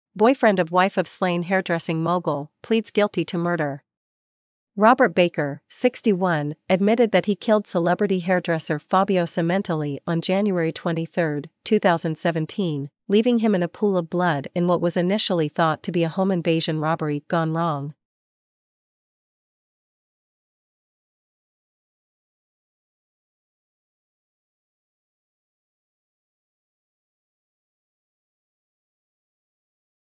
Boyfriend of wife of slain hairdressing mogul pleads guilty to murder
Robert Baker, 61, admitted that he killed celebrity hairdresser Fabio Sementilli on Jan. 23, 2017, leaving him in a pool of blood in what was initially thought to be a home-invasion robbery gone wrong.